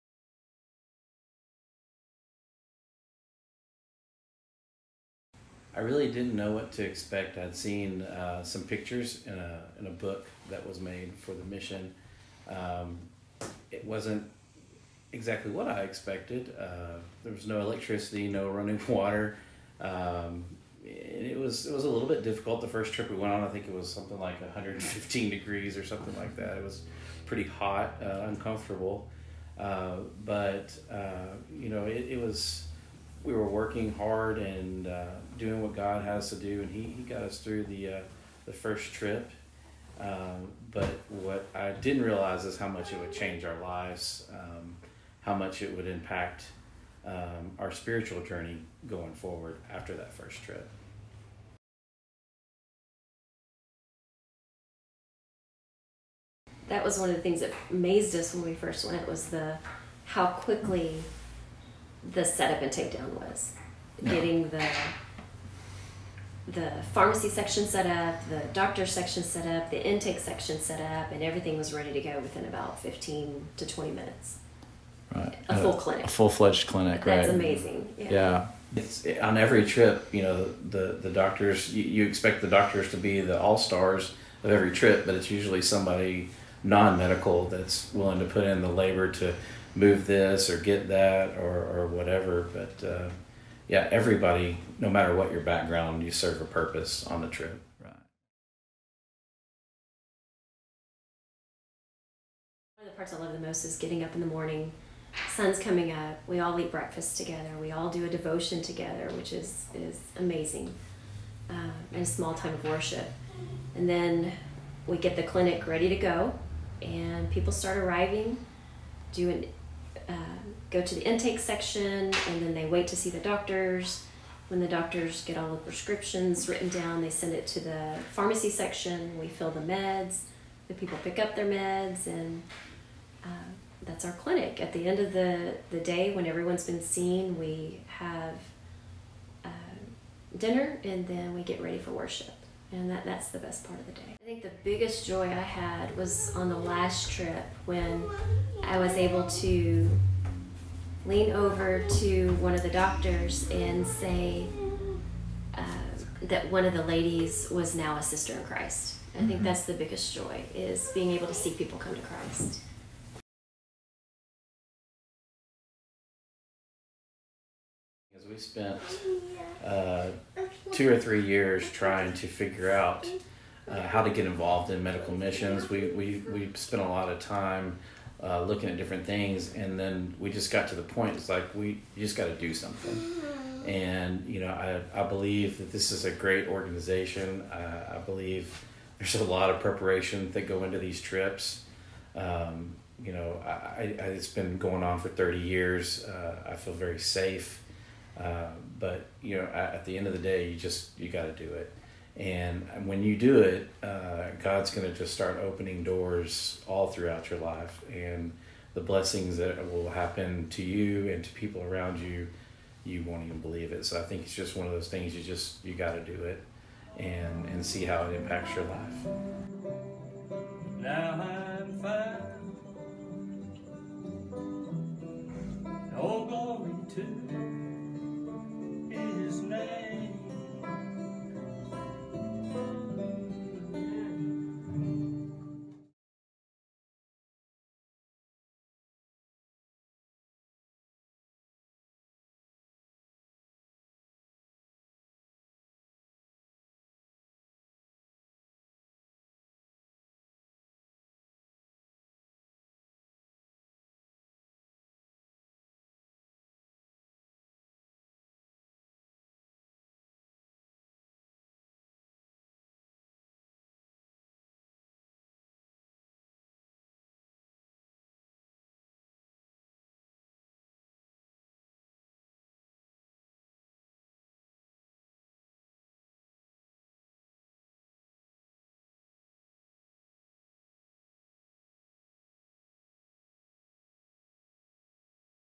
Testimonial- Fiesta 2020
Brian and Camri Dishman share their experience with the mission work in Mexico. Brian is a Respiratory Therapist, and Camri is a Registered Nurse. They have worked in the medical field for 20 years.